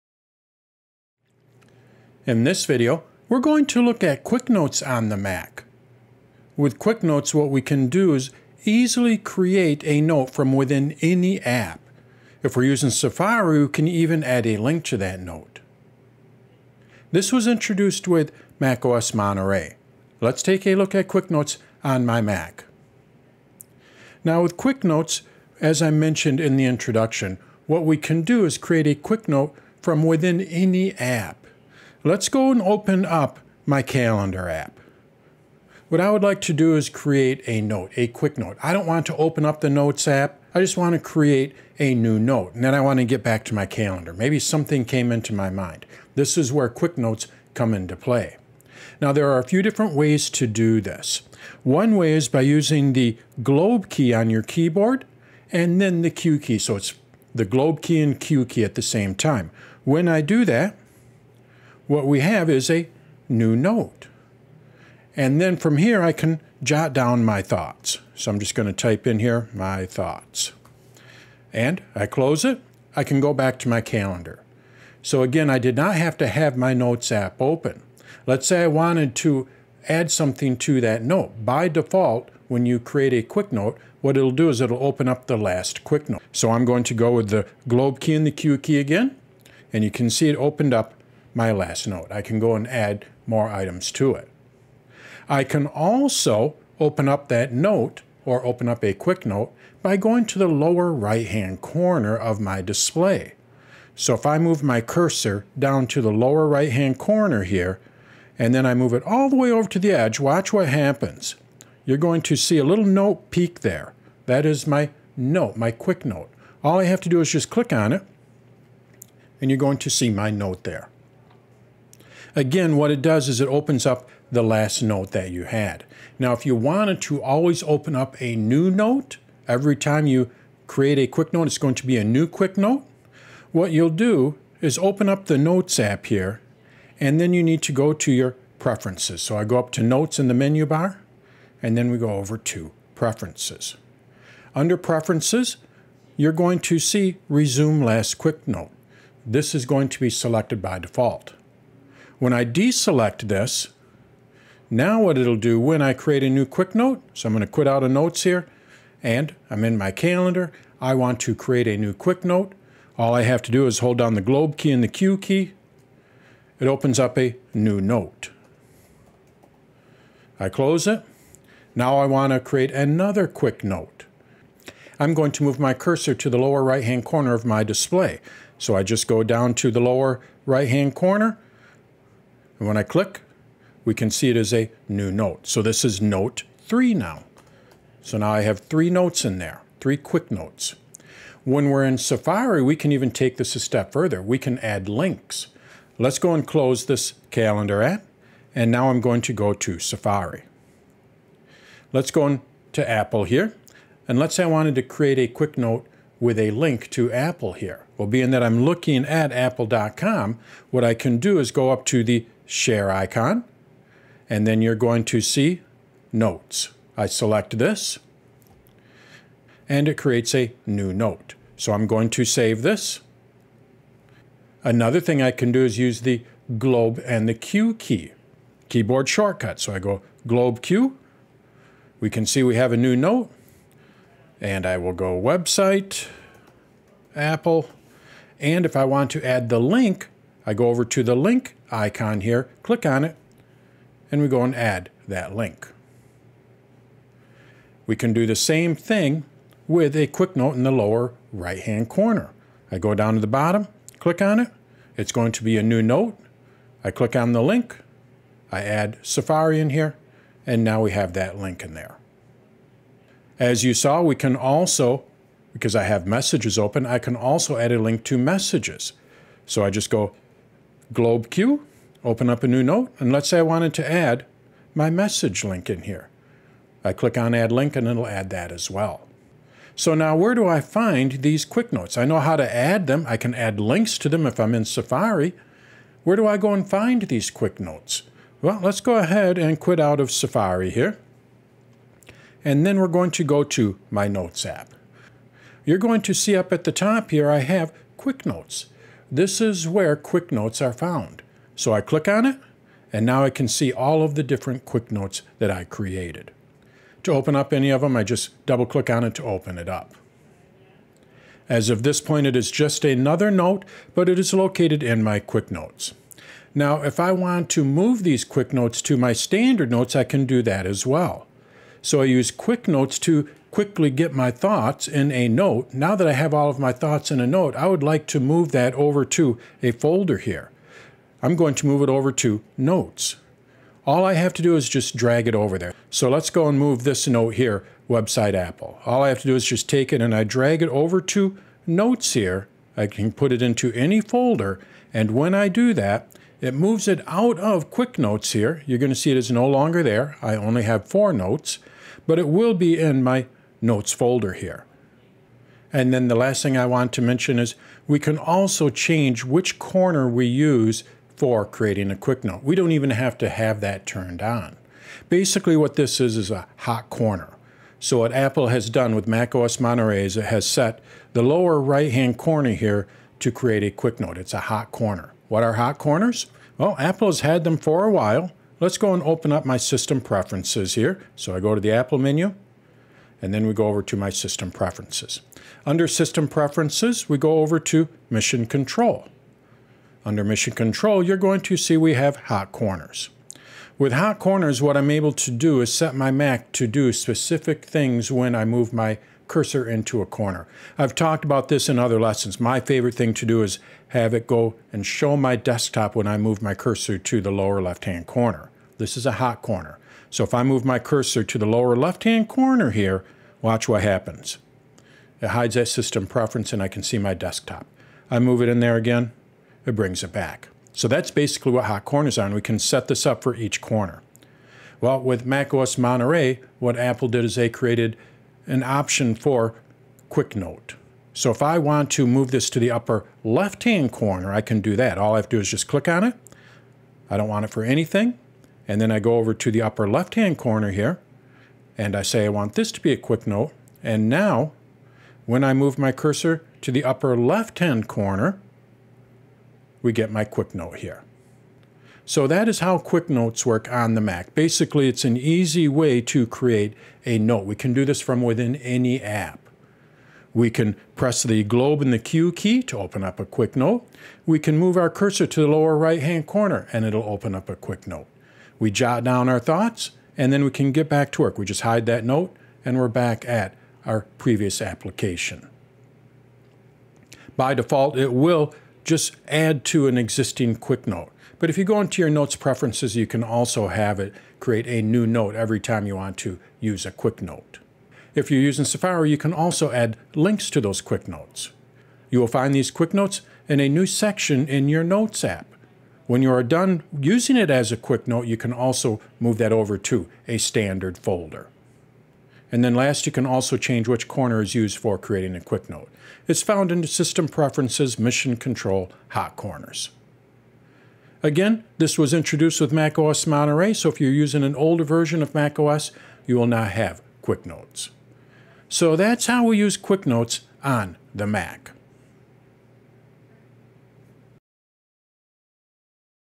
Create Quick Notes on the Mac
Learn how to create a Quick Note from within any app on the Mac.

With Quick Notes, you can create a new note while within any app by using a keyboard shortcut or moving your cursor to the lower right corner of your display. You can then find all your Quick Notes in the Notes app. Learn about Quick Notes in this video for Notes on the Mac.